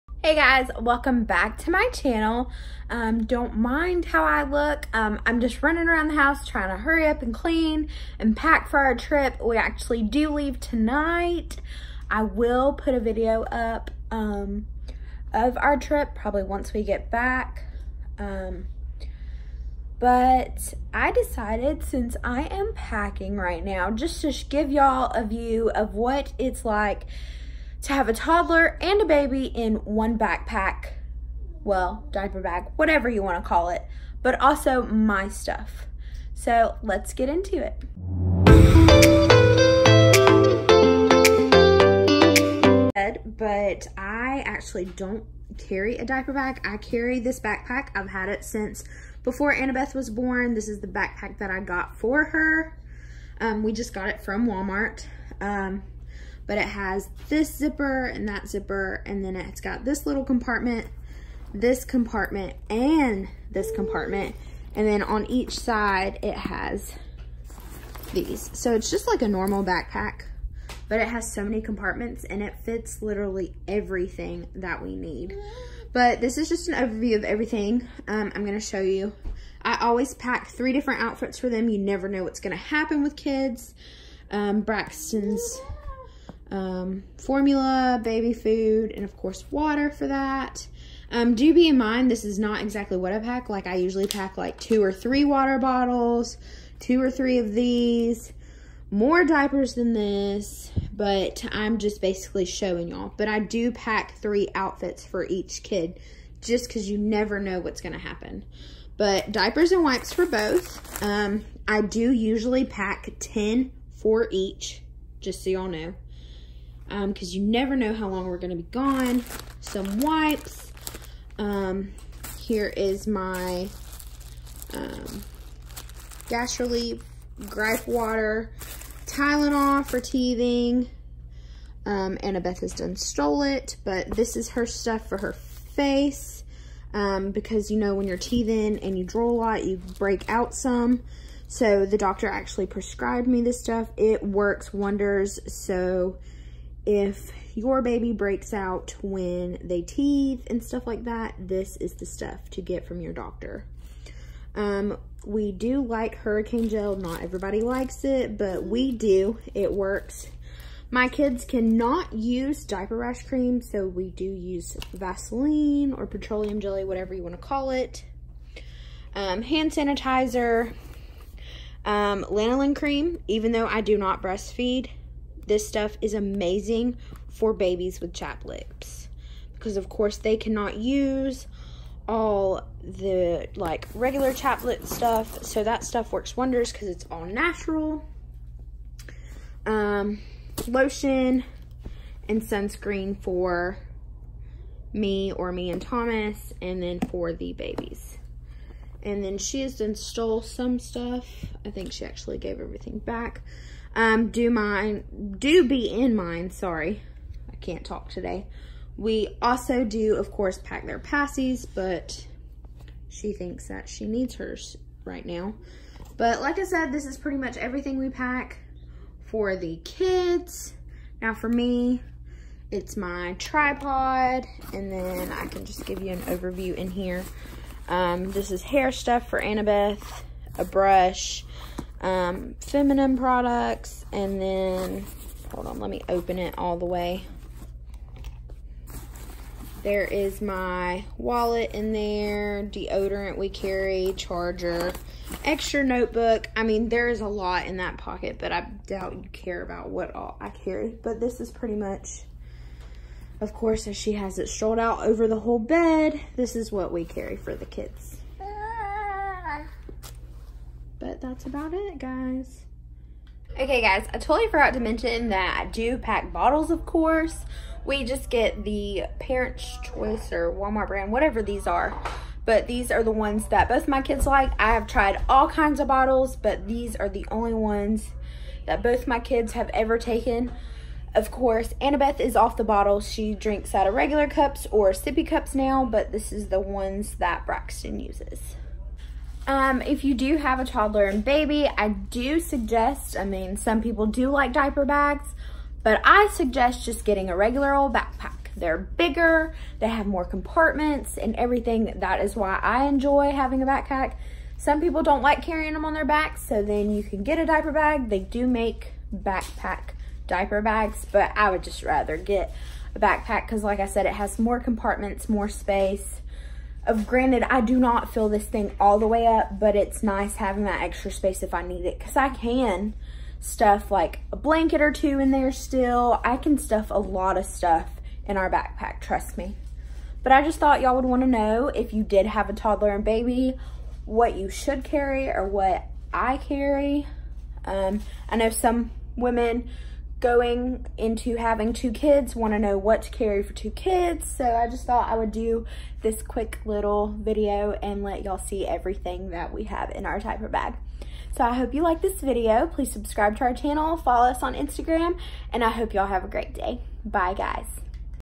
What’s in our Diaper Bag? Toddler + Baby / SAHM VLOG
We have 2 kids. Annabeth is 1 1/2 and Braxton is 6 months. This is a video of what we pack for them and mom with space left! Hope you enjoy this video!